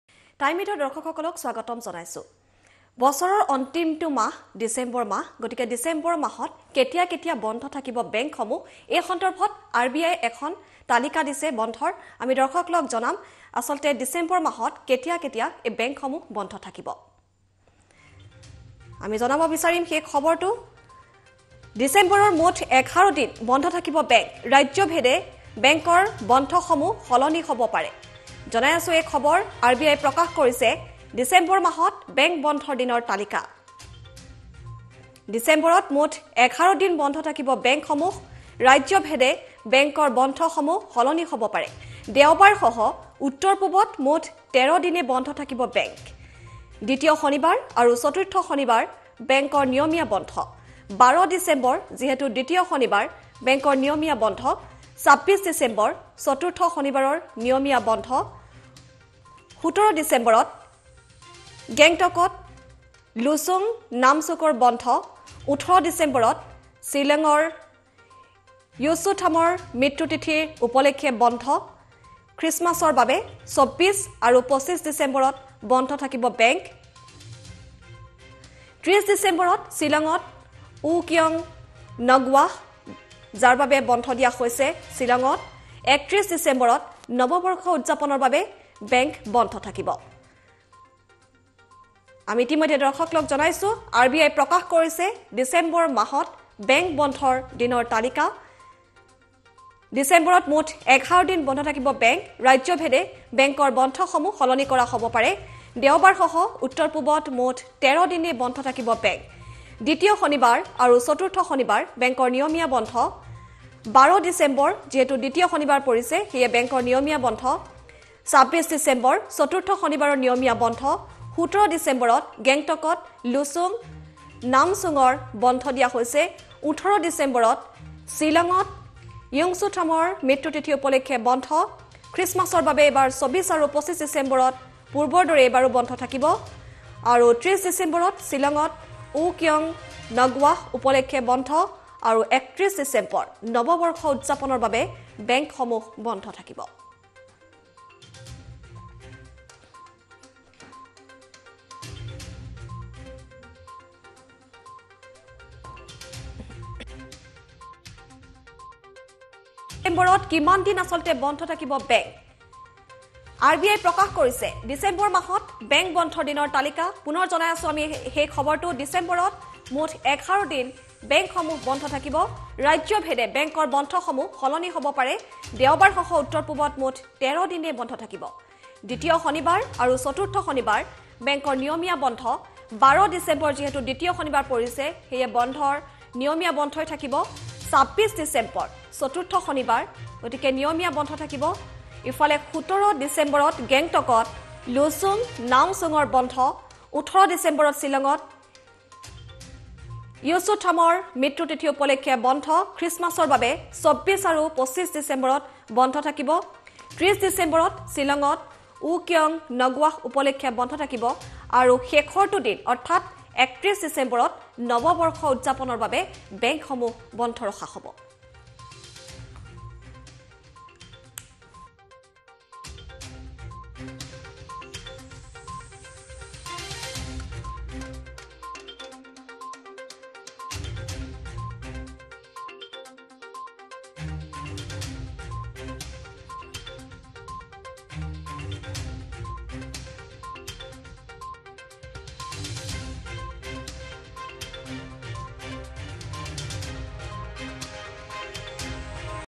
ডিচেম্বৰত কিমানদিন বন্ধ থাকিব বেংক?
ডিচেম্বৰত কিমানদিন বন্ধ থাকিব বেংক? জানি লওঁক সৱিশেষ